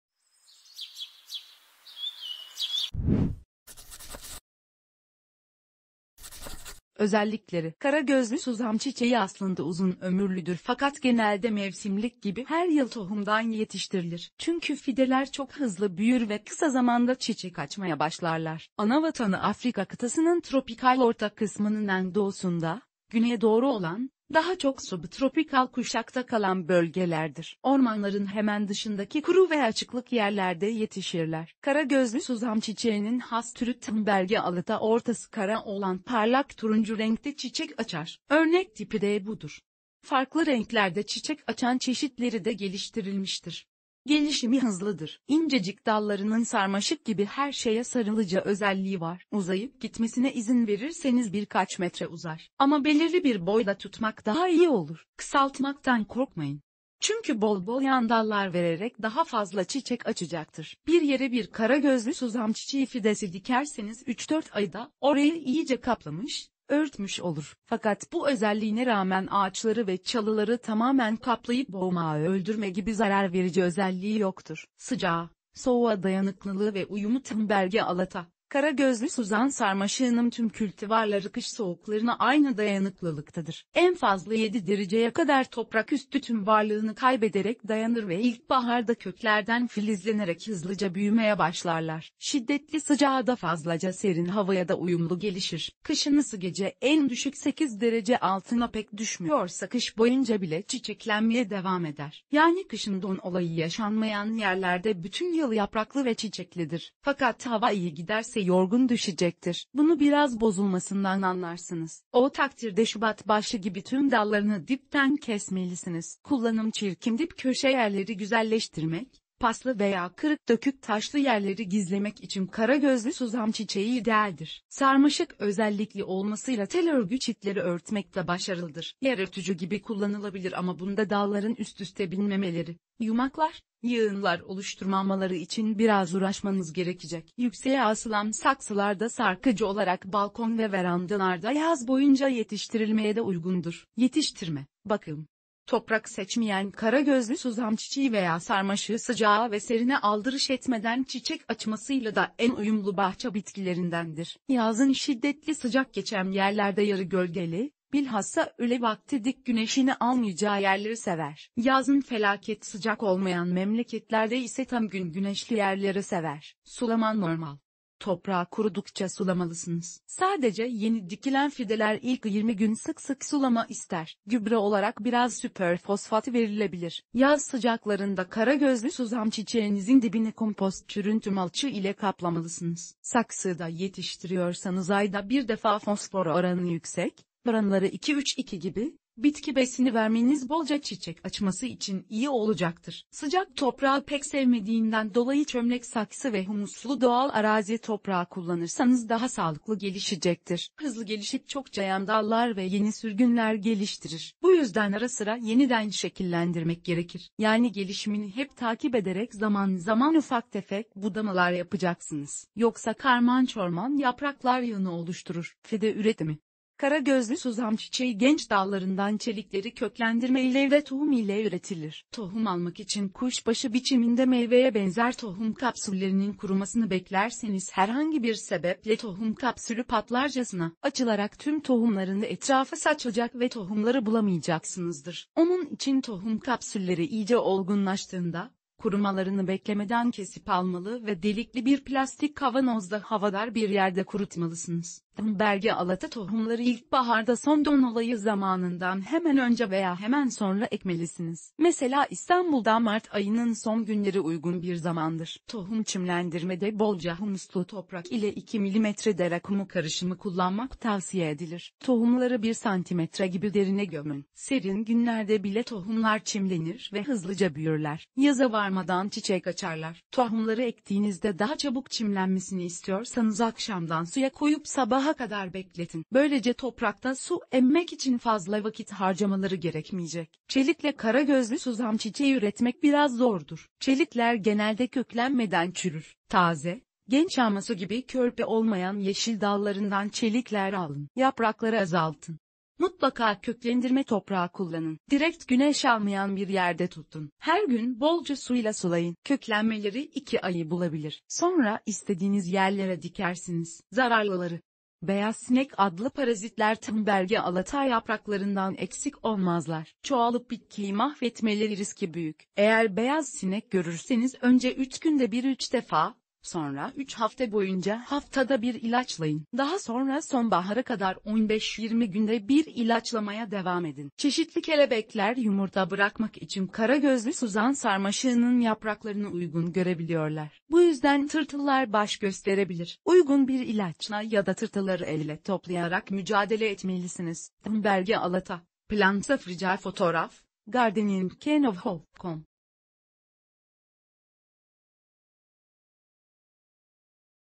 Kara gözlü Suzan çiçeği
Thunbergia alata, kara gözlü Suzan çiçeği çirkin dip köşe yerleri güzelleştirmek, paslı ve kırık dökük taşlı yerleri gizlemek için idealdir.